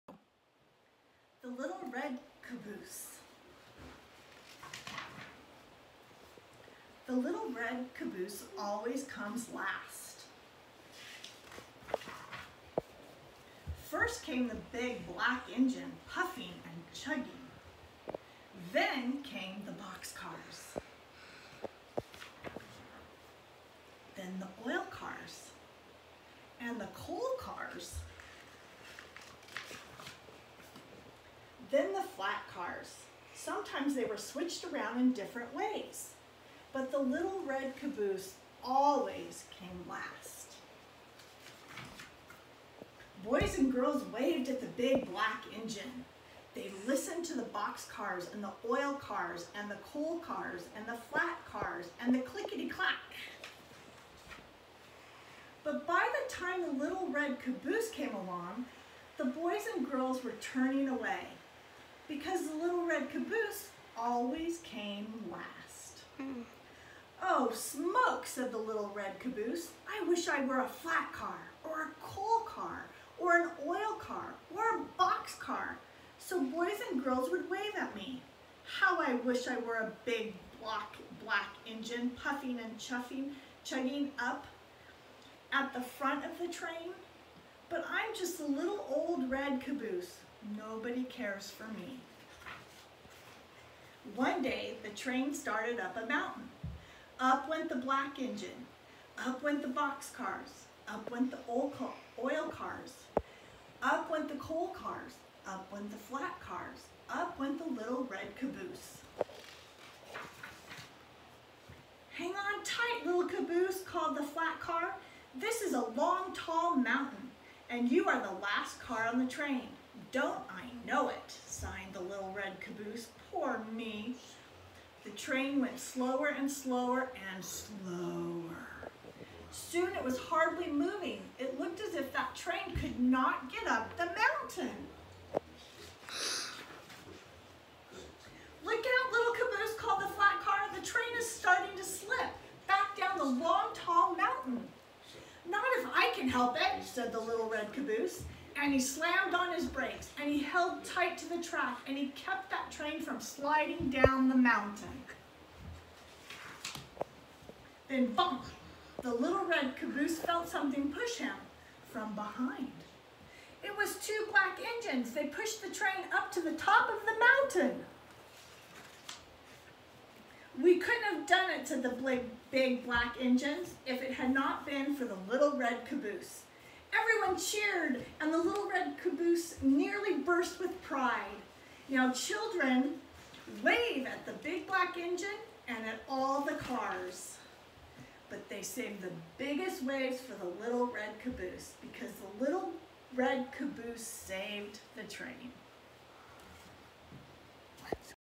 Principal Mrs. Dixon Reads “The Little Caboose”
Principal Mrs. Dixon reads a story to her Tovashal Tigersharks. There is fun in education still to be had virtually!